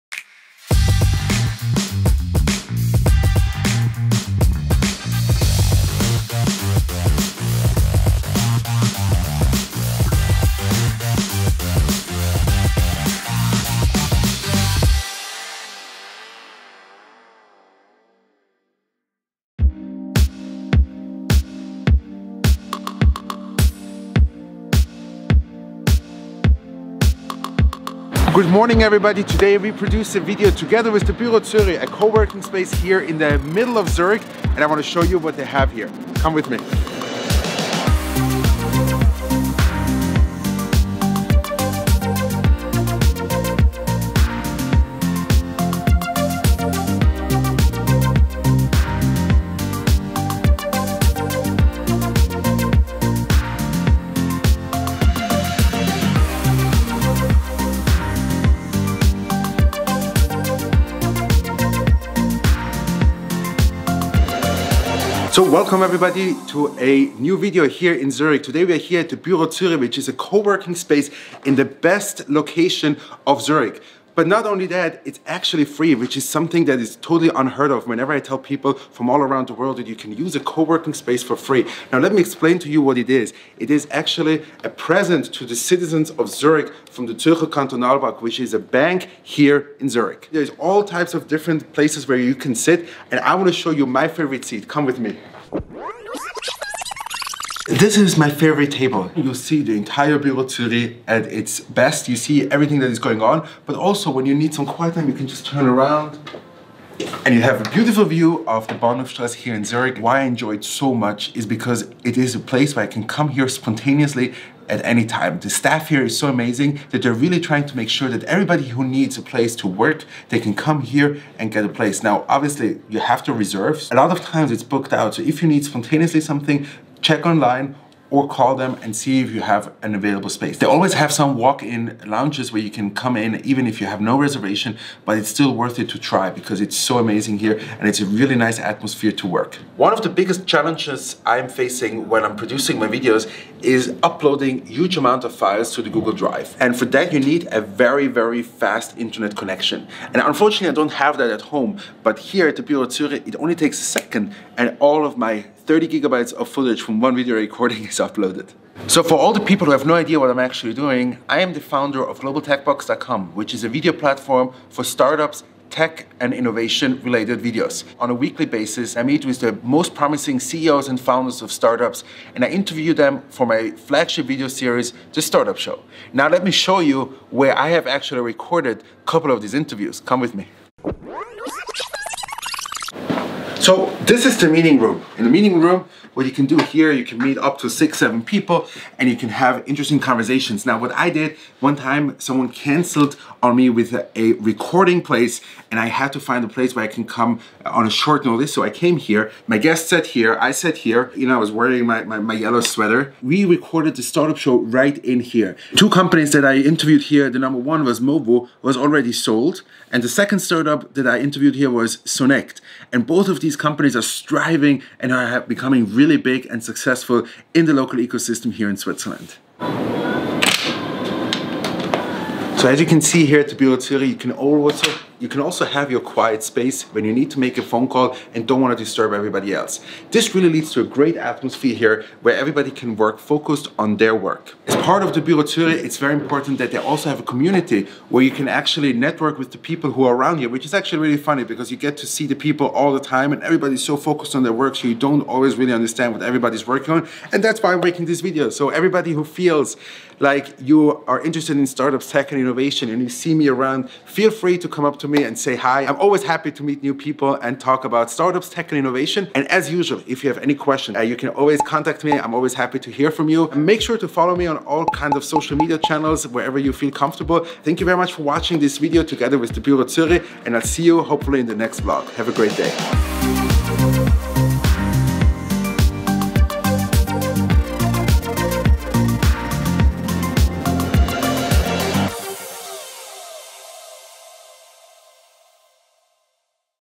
Why I Love Going to the BüroZüri / Vlog #32
"Köpfe aus dem Büro Züri"

Working from home is a very popular alternative to going to an office every day. The BüroZüri  is my co-working space - more inspiring and practical than many others! Watch the video to find out why I choose to go there. And next time you decide to venture to the BüroZüri, make sure to hit me up! I always enjoy meeting new people. 😀